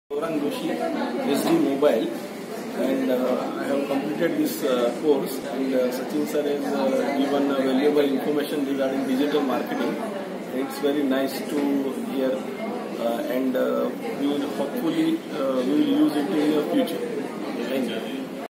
Testimonial by Gaurang Doshi - SDE(Mobile) BSNL India - 16 Nov, 2018
Testimonial for Digital Dojo - two days digital marketing workshop at BSNL, Mumbai